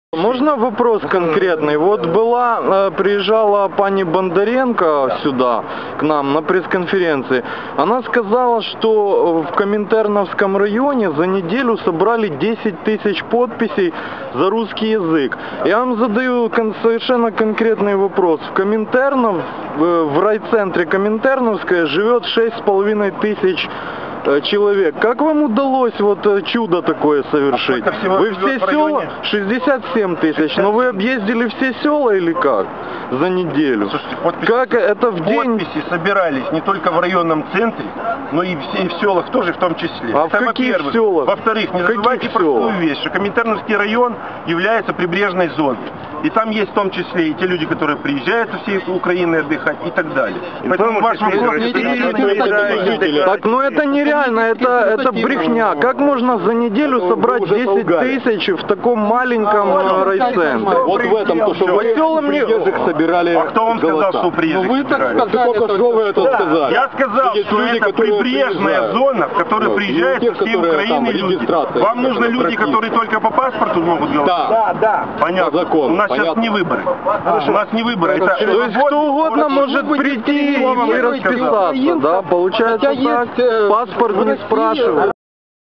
Одесский регионал отмазал Елену Бондаренко
Депутат Одесского облсовета от Партии регионов отмазал вруниху Елену Бондаренко